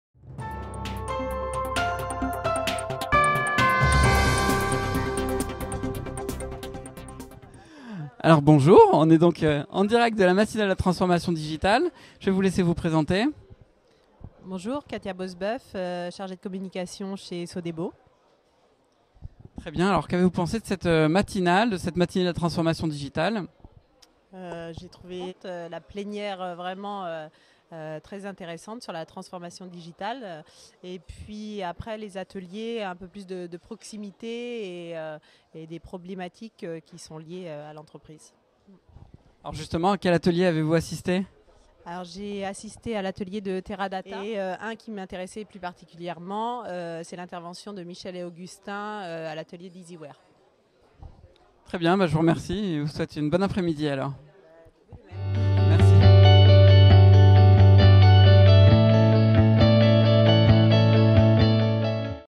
Katia Bosseboeuf, Sodebo, dans la BAQ de la Matinale de la Transformation Digitale
Katia Bosseboeuf nous répond dans la Boite à questions de la Matinale !
Pour suivre des experts en Digital, rendez-vous sur le Blog Big Data & Digital